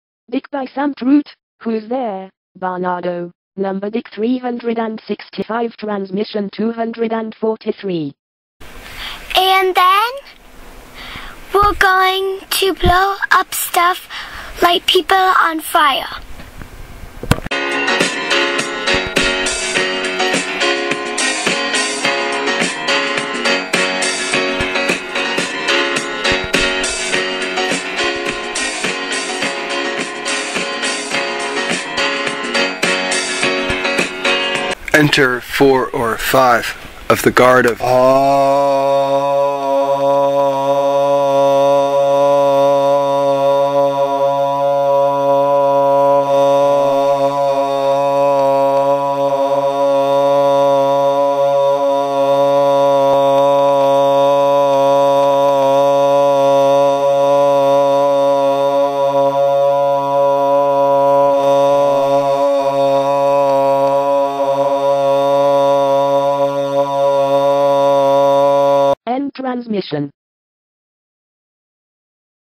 DICK 243
This is one of 365 transmissions (22.Nov.12 - 22.Nov.13) of DICK, a book by Sam Truitt. For more search "DICK: An Oblique Kennedy Conspiracy Countdown."